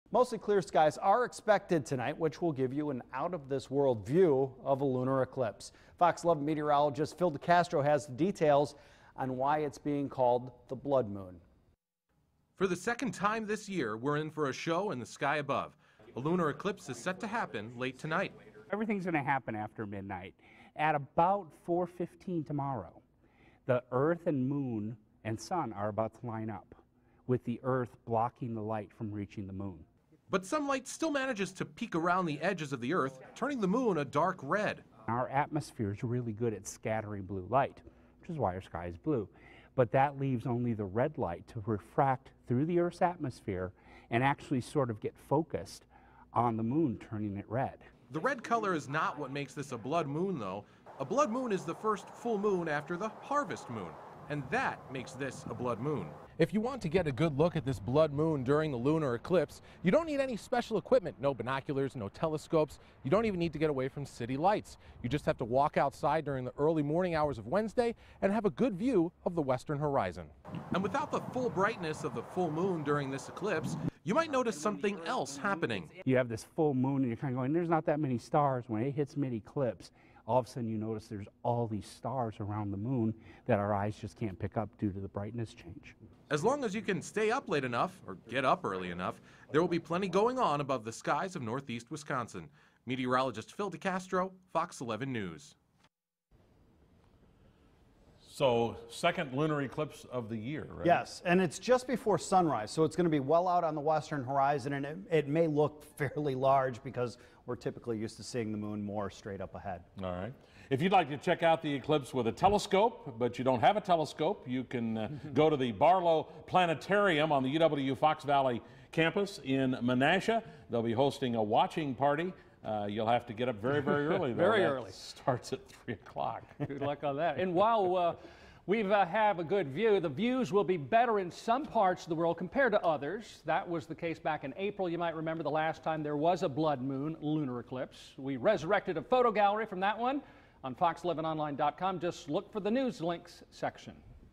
Lunar Eclipse Preview
A lunar eclipse is set to happen during the early morning hours of Wednesday, October 8th, 2014